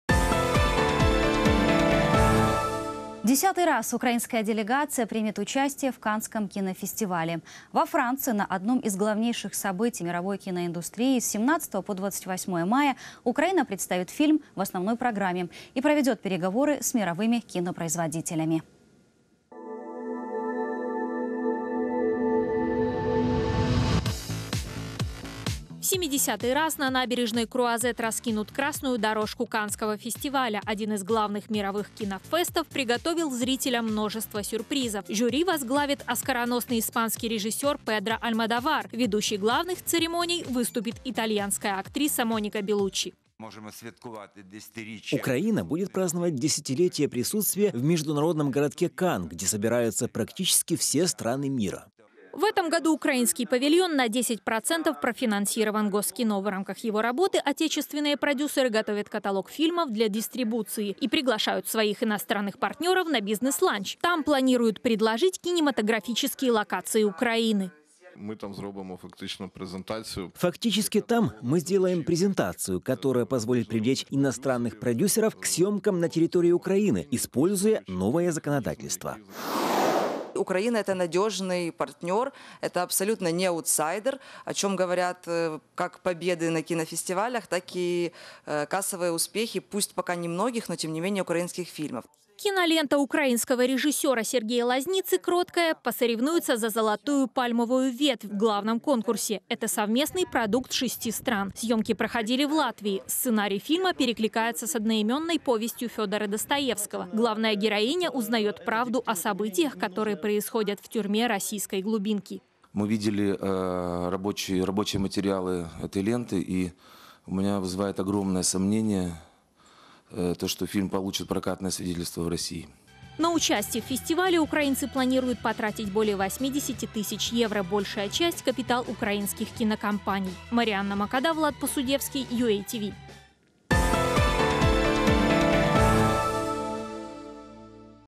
Украина примет участие в Каннском кинофестивале
Во Франции на одном из главнейших событий мировой киноиндустрии, с 17 по 28 мая, Украина представит фильм в основной программе и проведёт переговоры с мировыми кинопроизводителями.